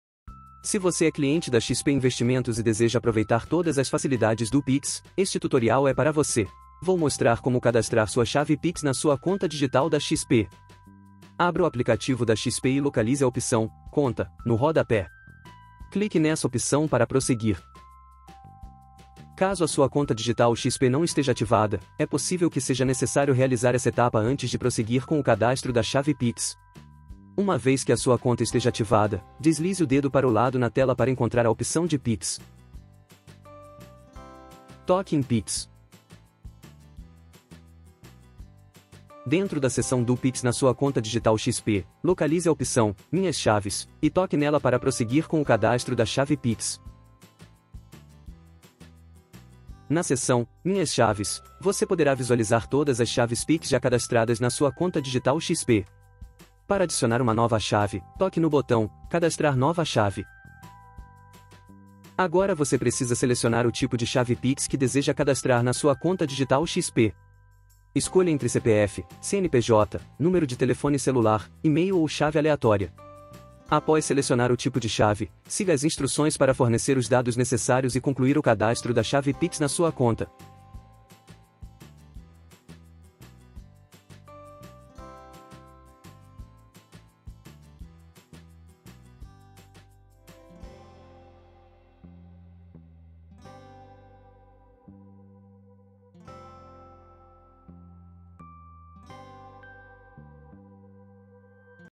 Como CADASTRAR CHAVE PIX na CONTA DIGITAL XP
Neste vídeo, vamos te mostrar o passo a passo completo de como cadastrar a chave Pix na sua conta digital XP Investimentos.

O Pix é um sistema de pagamentos instantâneos que tem revolucionado as transações financeiras, trazendo praticidade e agilidade para os usuários.

Se você possui uma conta digital XP Investimentos e deseja aproveitar os benefícios do Pix, como transferências rápidas e seguras, é fundamental realizar o cadastro da chave Pix.

A chave Pix pode ser o seu CPF, e-mail, telefone ou até mesmo uma chave aleatória, e será utilizada para identificar a sua conta nas transações.